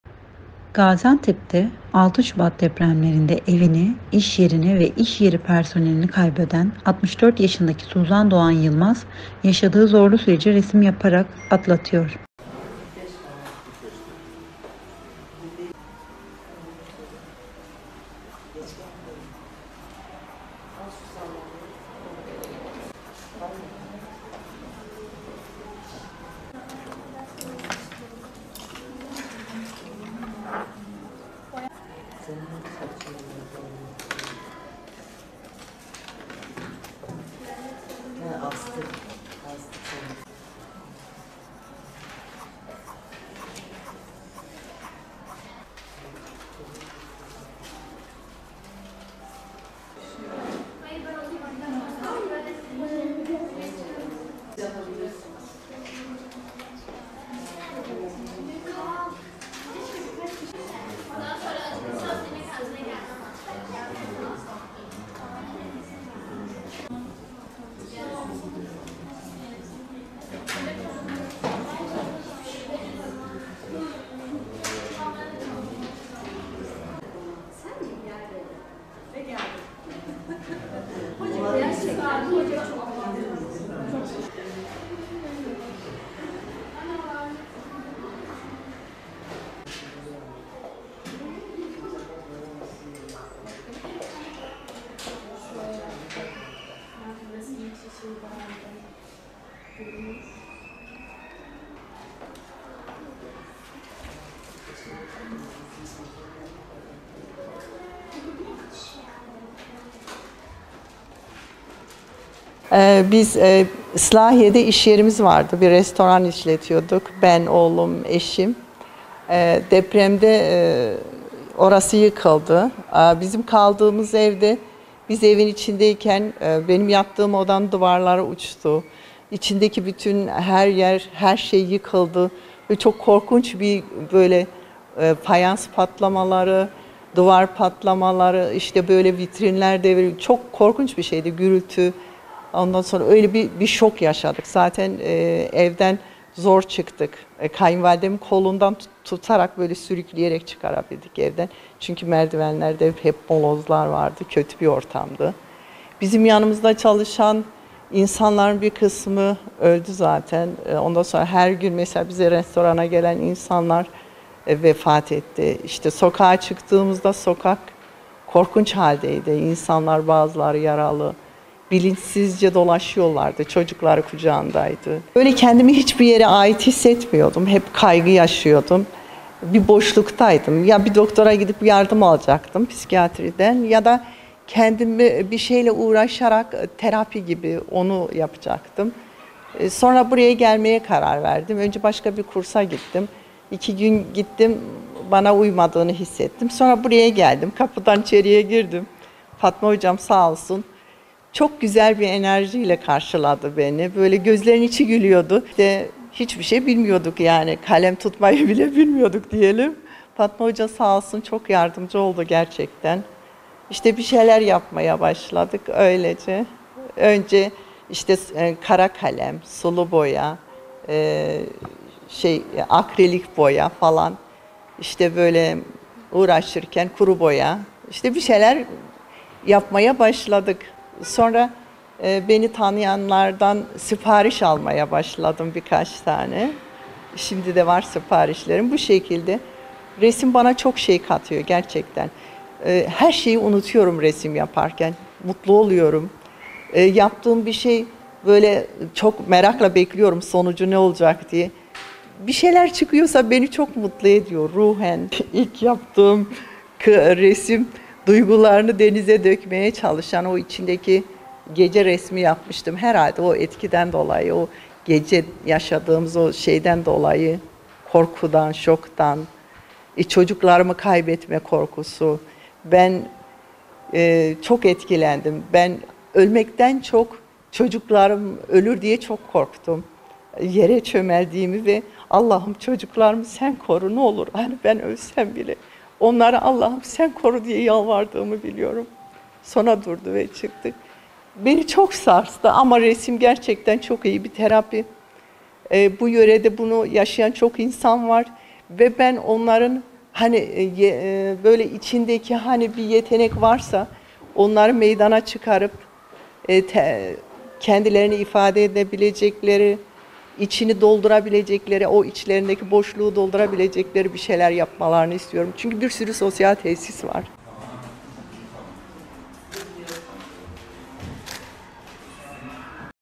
Depremzede Kadın, Depremin Acısını Resimle Aşıyor
Gaziantep’te, 6 Şubat depremlerinde evini, iş yerini ve iş yeri personelini kaybeden 64 yaşındaki Suzan Doğan Yılmaz, yaşadığı zorlu süreci resim yaparak atlatıyor